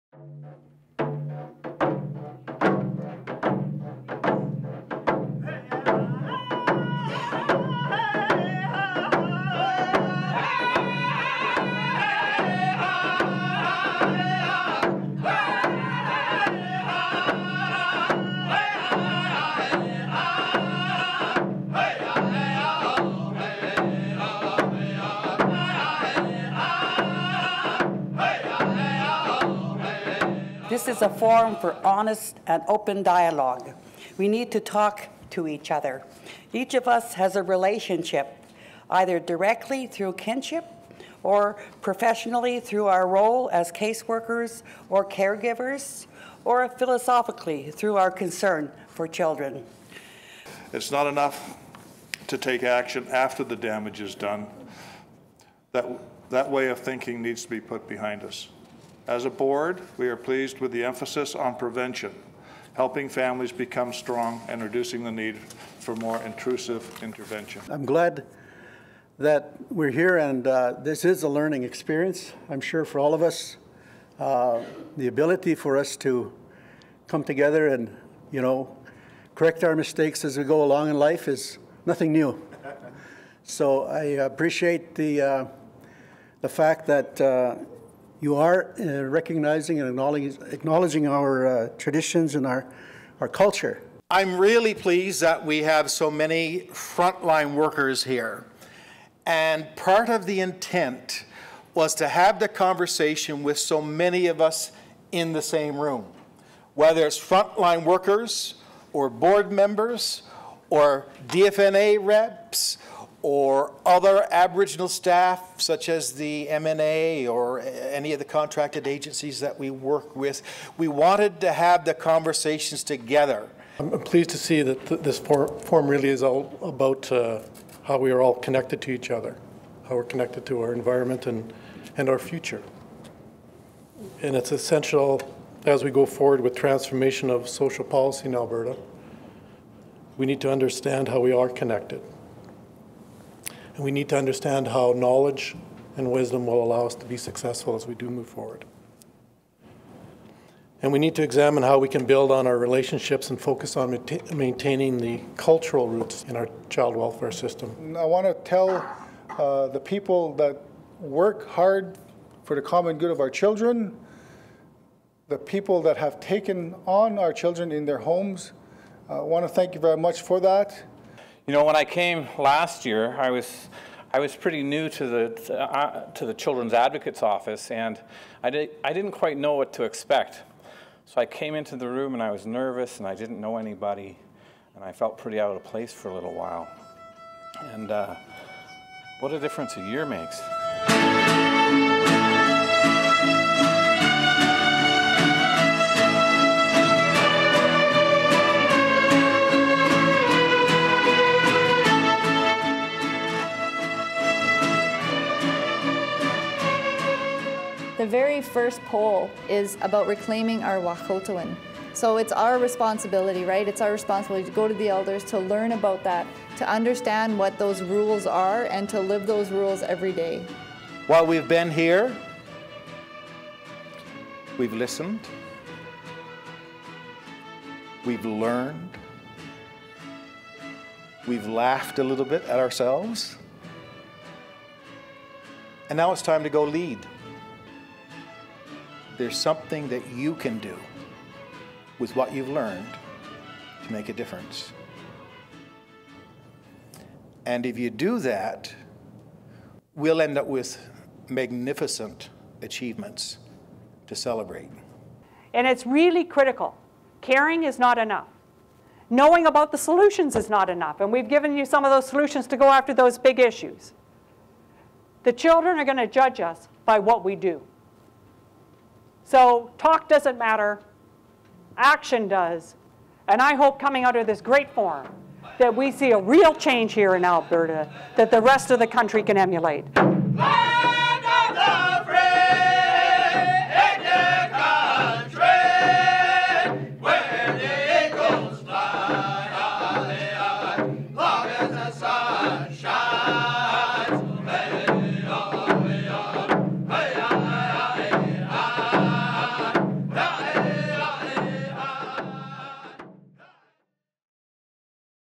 Aboriginal Forum II - Growing Our Relations: Wahkohtowin
Human Services brought Aboriginal and non-Aboriginal partners together on May 1 and 2 to build relationships and share insights into helping Aboriginal children, youth and families.